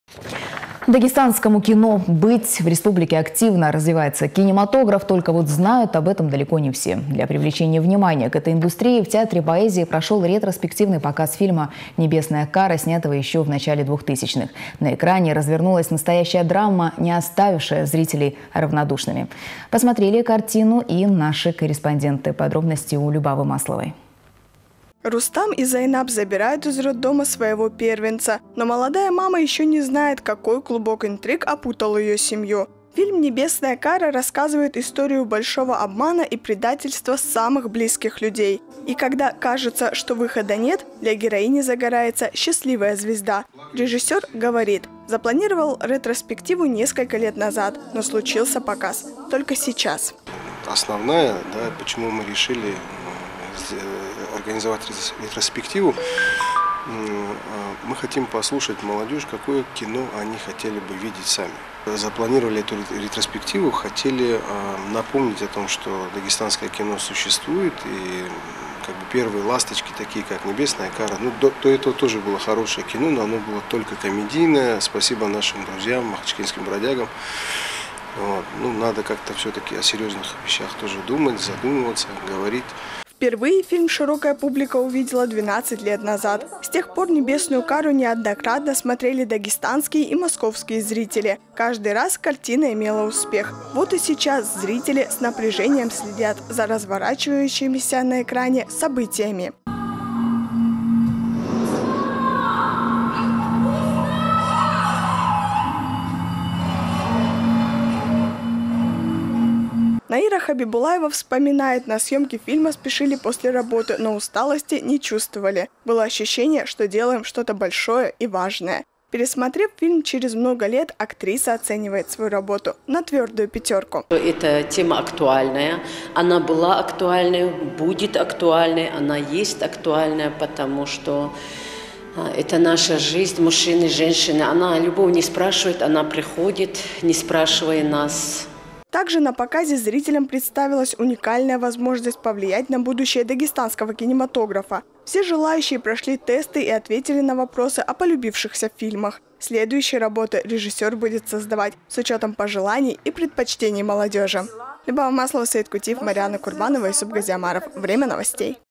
«Небесная кара». Нашему кино быть
Телеканал РГВК Дагестан, сюжет Л.Масловой
Дагестанскому кино быть. В республике активно развивается кинематограф, только вот знают об этом не все. Для привлечения внимания к этой индустрии в Театре поэзии прошел ретроспективный показ фильма «Небесная кара», снятого еще в начале 2000-х. На экране развернулась настоящая драма, не оставившая зрителей равнодушными.